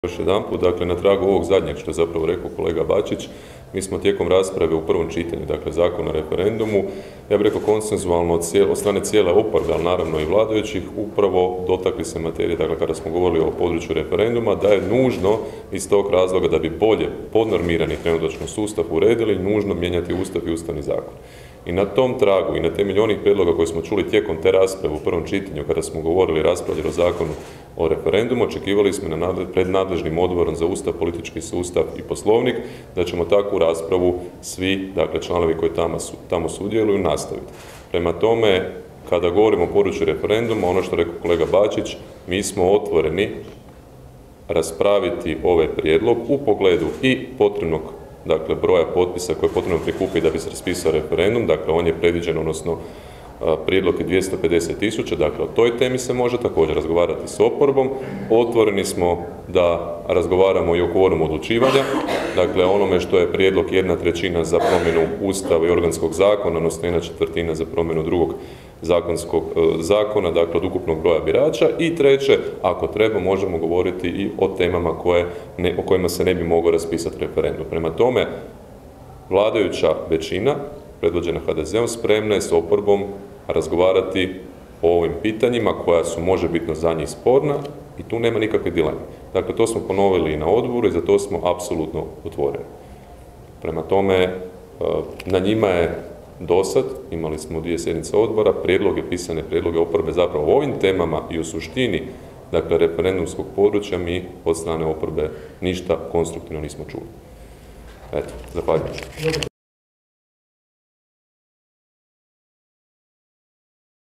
Damir Habijan: "O nekim stvarima spremni smo razgovarati s oporbom"
Na konferenciji za medije kluba zastupnika HDZ-a održanoj u Saboru o temi "Oporbeno politiziranje o izmjenama Ustava i Ustavnog zakona o Ustavnom sudu" govorili su zastupnici Branko Bačić i Damir Habijan.